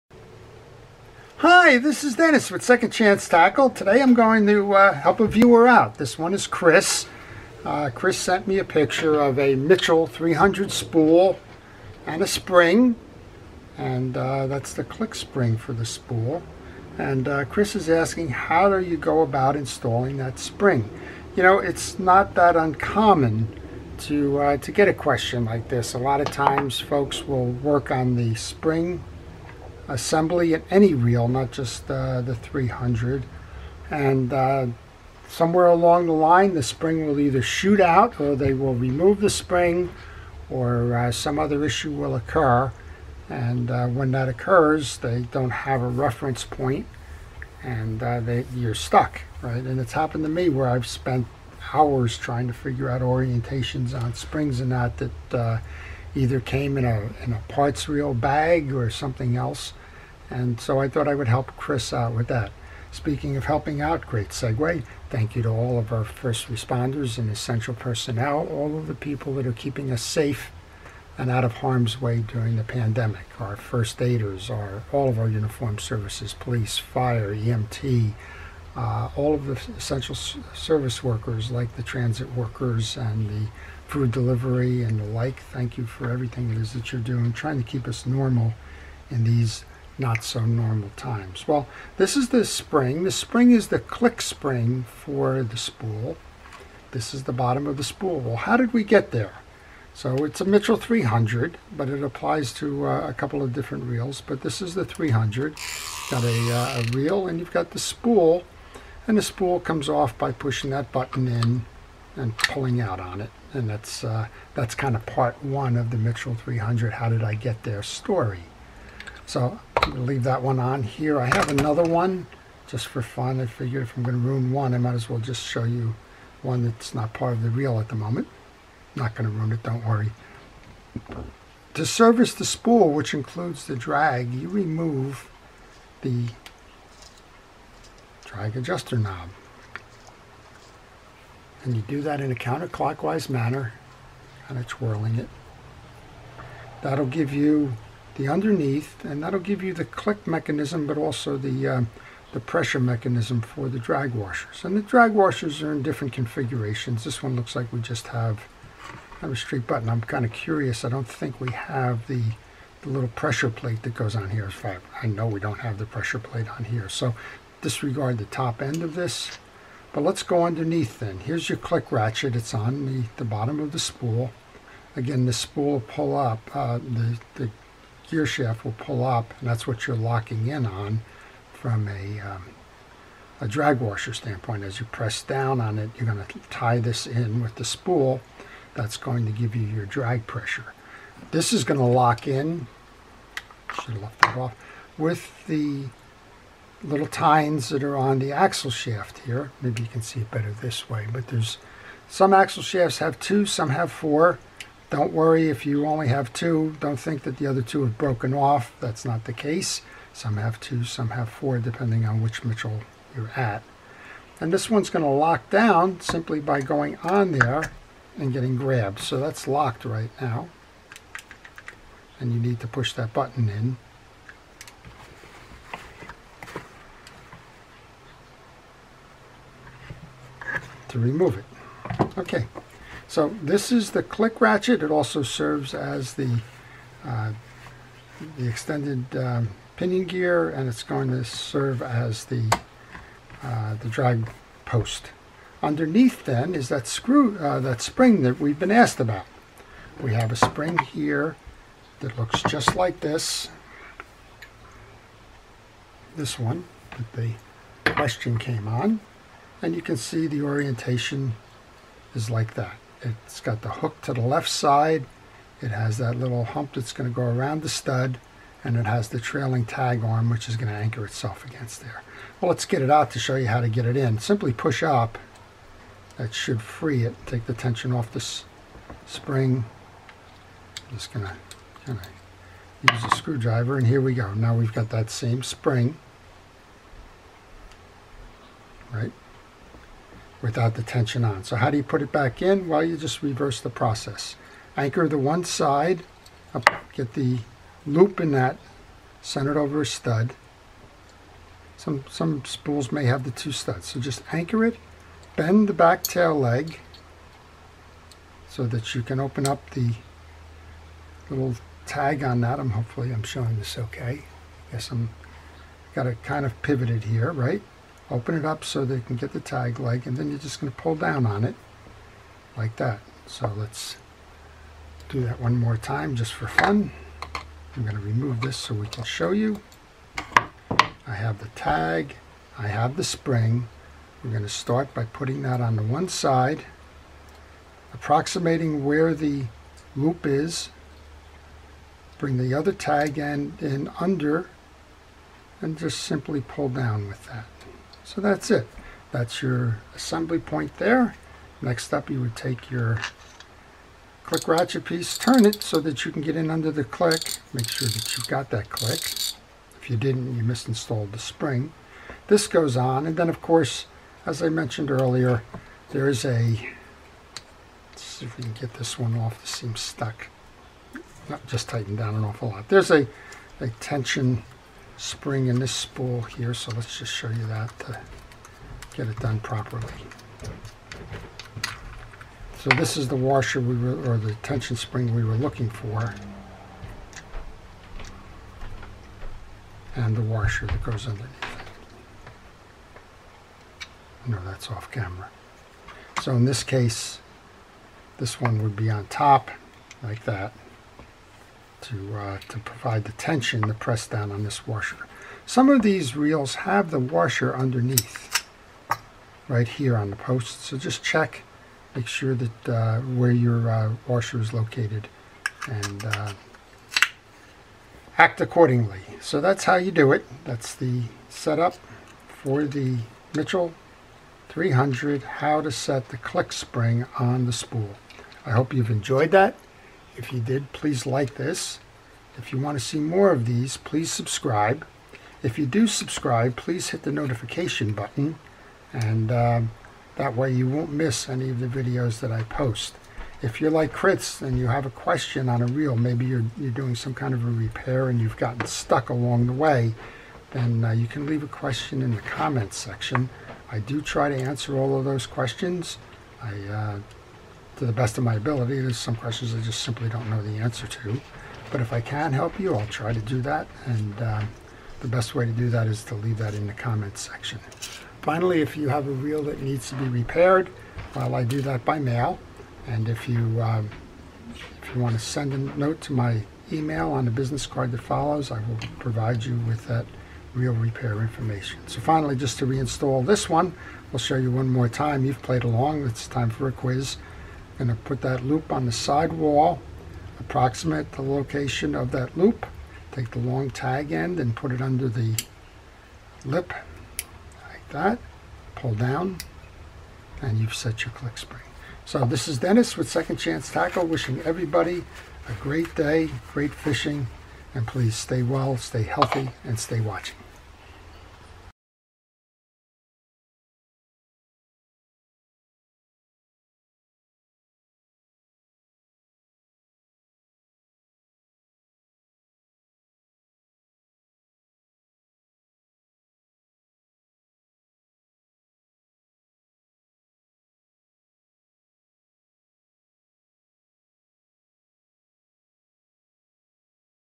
How to set the spool click spring on a Mitchell 300 spin fishing reel
This video shows how to set the spool click spring on a Mitchell 300 spool.  It is in answer to a viewer's question and shows the step by step installation process and explains how it works.
I have been servicing fishing for over 20 years and am sharing my experiences here.
Questions?  Comments?  I reply to all.